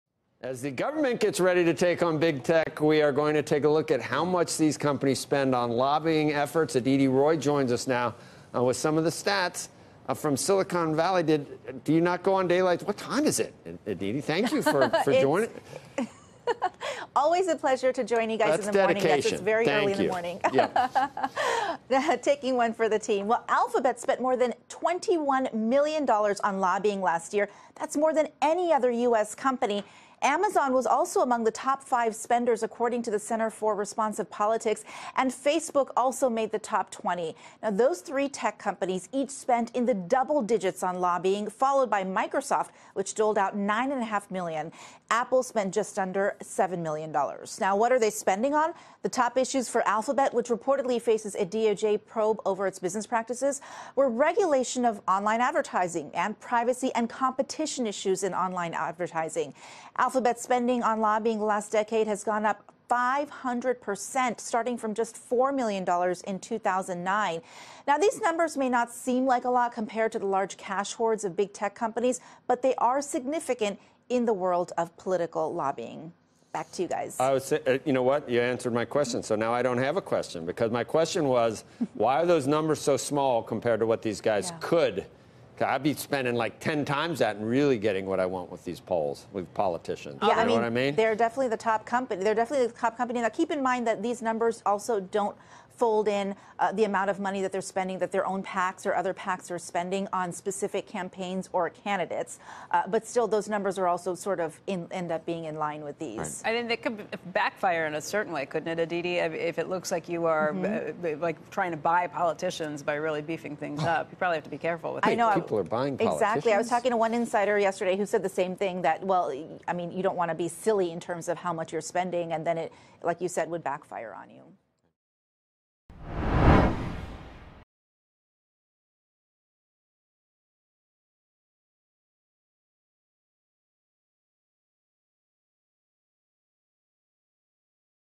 Here's how much big tech is spending on lobbying efforts
As the government gets ready to take on big tech, CNBC's Aditi Roy looks at how much those companies spend on lobbying efforts.